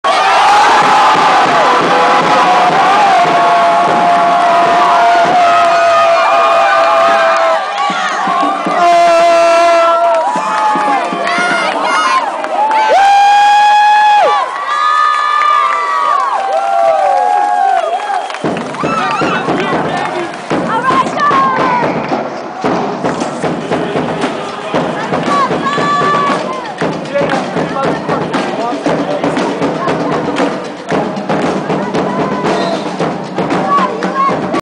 USF Basketball- Watkins Throwns down a HUGE dunk!
Jay Watkins, aka Freshman Phenom throws down a MONSTER dunk at USF vs. Sonoma. Los Locos goes NUTTS.

(sorry I only caught a peice of it)